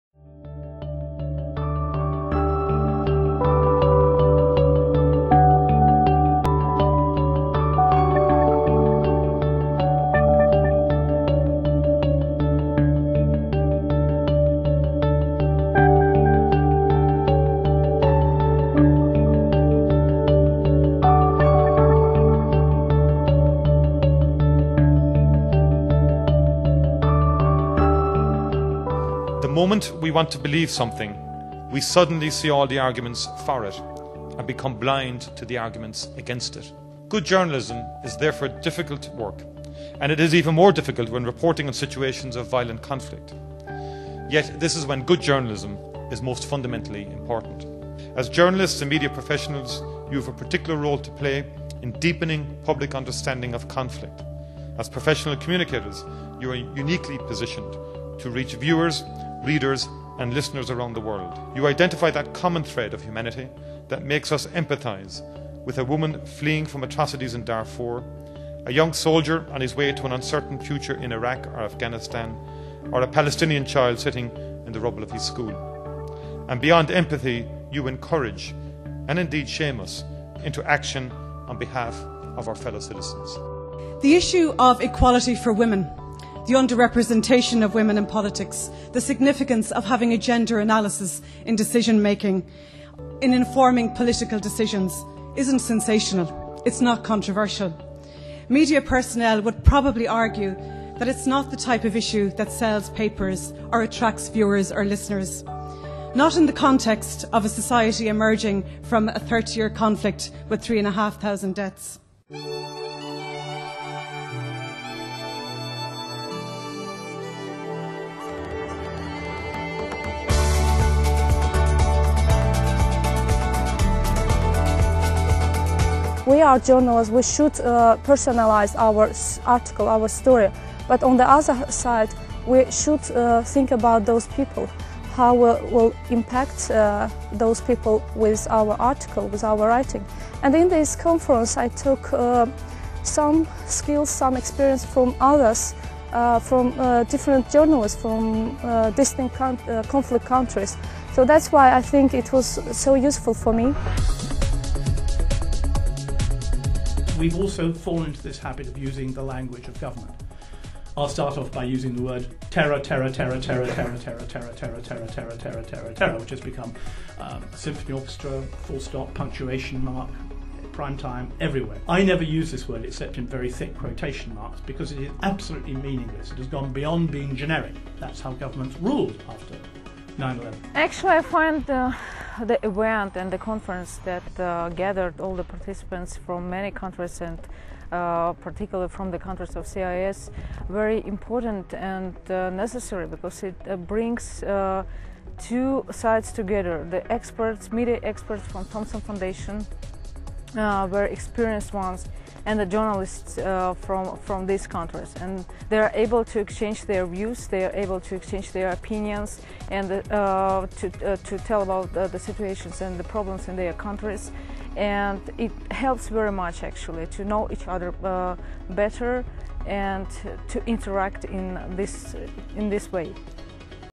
Bringing together journalists from either side of the EU's borders
Workshops in Ireland, which the the Thomson Foundation helped to organise, brings together reporters together from inside and outside the EU.